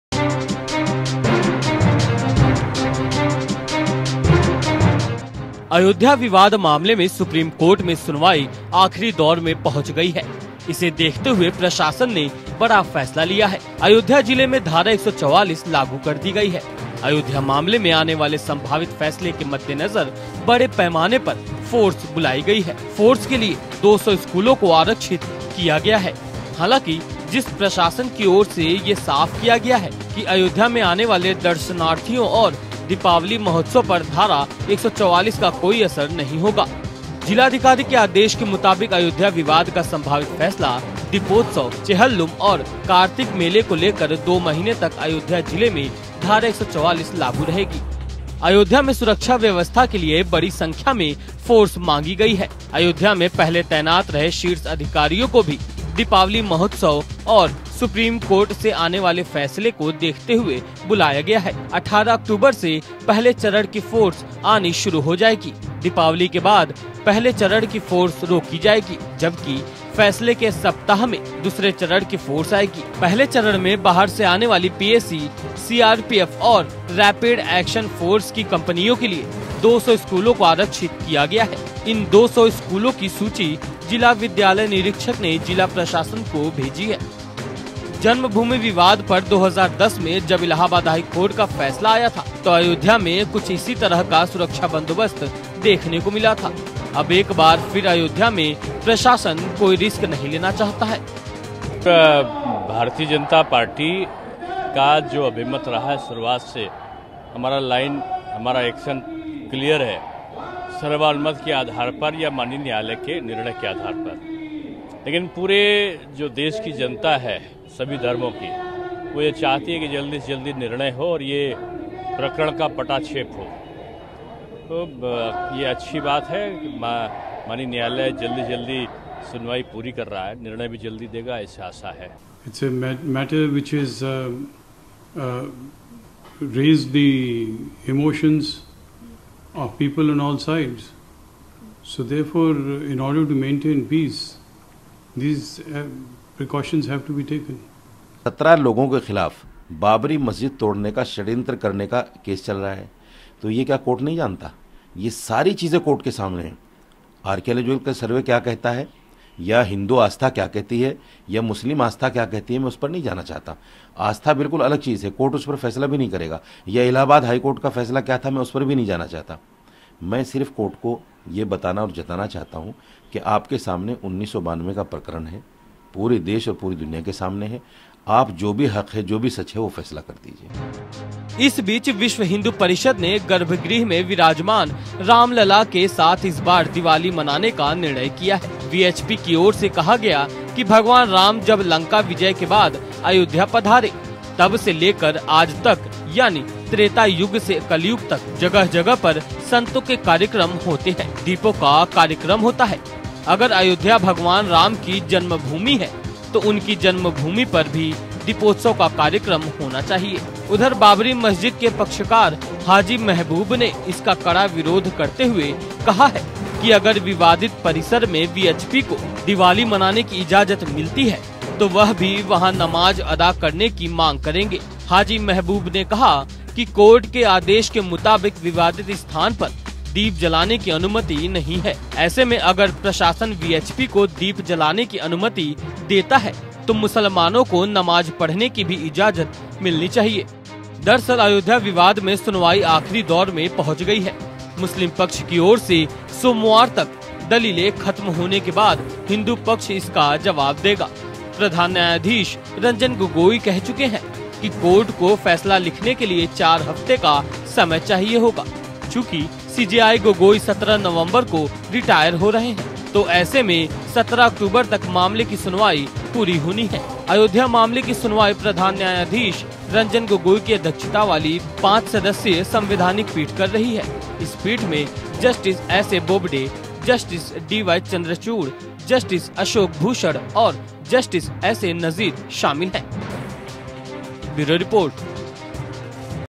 अयोध्या विवाद मामले में सुप्रीम कोर्ट में सुनवाई आखिरी दौर में पहुंच गयी है।
अयोध्या विवाद मामले में सुप्रीम कोर्ट में सुनवाई आखिरी दौर में पहुंच गयी है। इसे देखते हुए प्रशासन ने बड़ा फैसला लिया है। अयोध्या जिले में धारा 144 लागू कर दी गई है। अयोध्या मामले में आने वाले संभावित फैसले के मद्देनजर बड़े पैमाने पर फोर्स बुलाई गई है। फोर्स के लिए 200 स्कूलों को आरक्षित किया गया है। हालांकि जिसा प्रशासन की ओर से ये साफ किया गया है कि अयोध्या में आने वाले दर्शनार्थियों और दीपावली महोत्सव पर धारा 144 का कोई असर नहीं होगा।